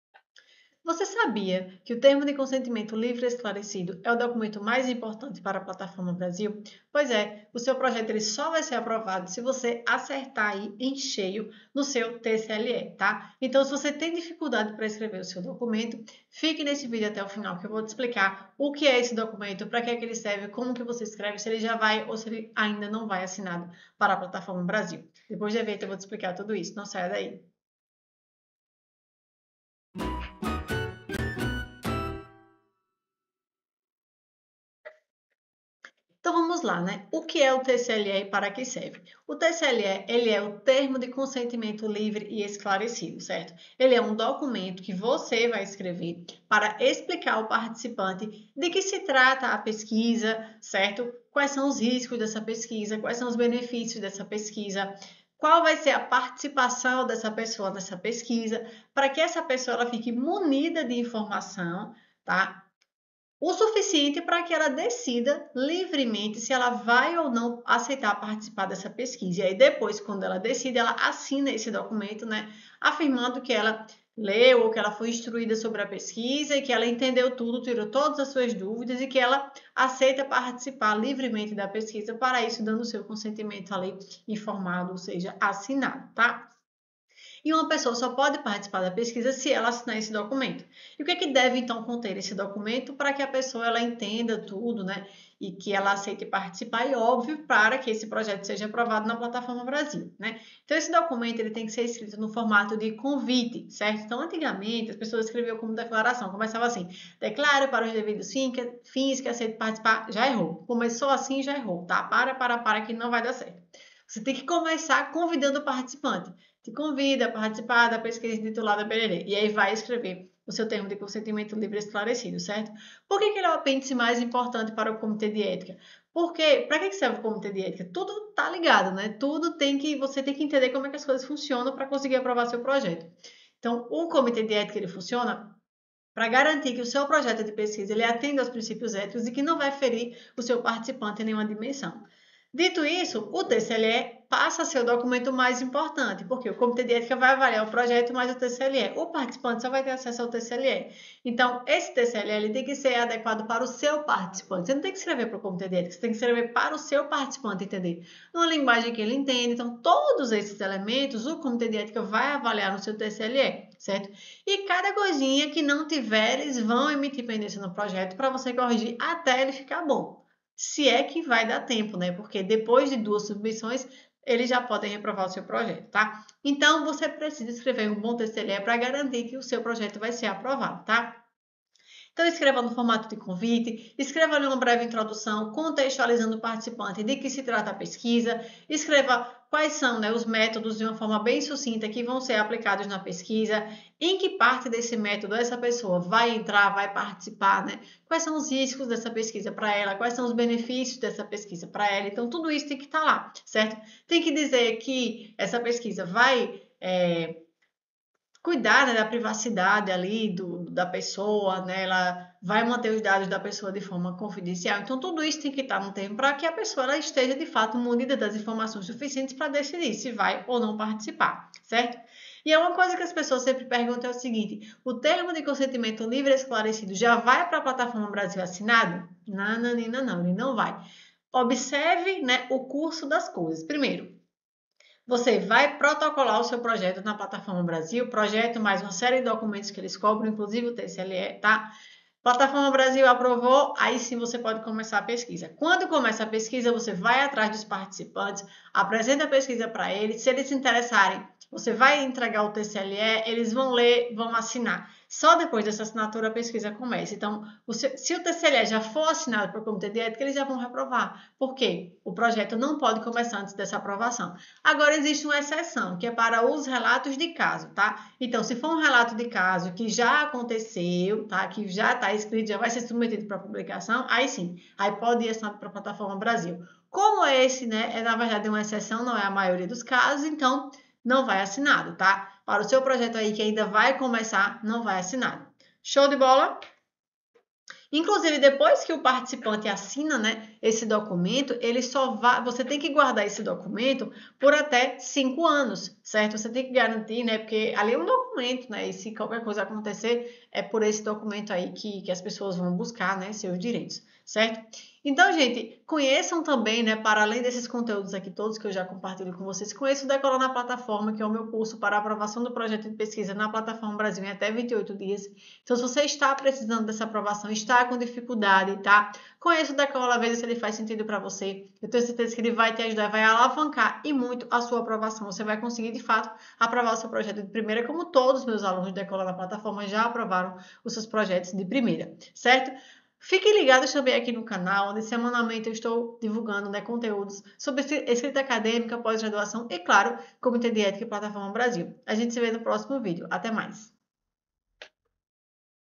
Como Escrever o Termo de Consentimento para Plataforma Brasil
🚀 Acesse agora o Decola na Plataforma e tenha sua aprovação rapida na Plataforma Brasil 🚀

 00:00 Introdução
 00:27 O que é o TSE
 01:10 Importância do TSE na pesquisa
 02:08 Estrutura do TSE
 03:06 Como apresentar o TSE ao participante
 04:06 Processo de aprovação na Plataforma Brasil
 05:10 Dicas finais e curso de apoio

Aprenda a elaborar o Termo de Consentimento Livre e Esclarecido (TSE) para a Plataforma Brasil de forma eficaz e completa. Neste vídeo, você descobrirá sua importância, estrutura e como apresentá-lo aos participantes, garantindo a aprovação do seu projeto de pesquisa. Essencial para pesquisadores que buscam conformidade ética e agilidade na aprovação.